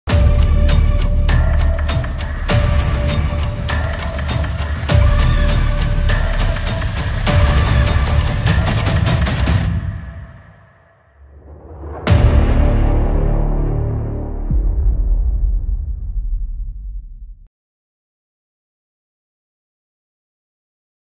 ZAPOWIEDŹ GALI WFMC PRO FIGHT NIGHT 8
Już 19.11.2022 w hali Arena Żagań odbędzie się kolejna gala organizacji WFMC PRO FIGHT NIGHT.

MUSISZ TAM BYĆ‼️🔥🎤

Video - @thebestframe2882